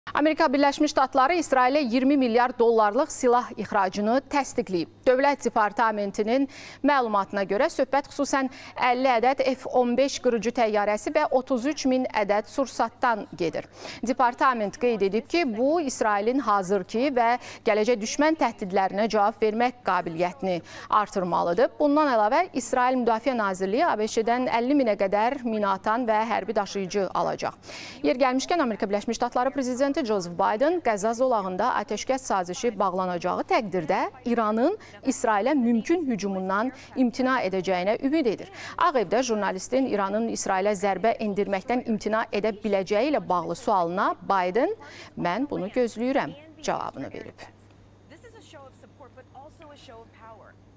ABŞ İsrailin arzusunu gerçəkləşdirdi: qırıcılar Tel-Əvivə göndərildi
ABŞ-DƏN İSRAİLƏ 20 MİLYARDLIQ DƏSTƏK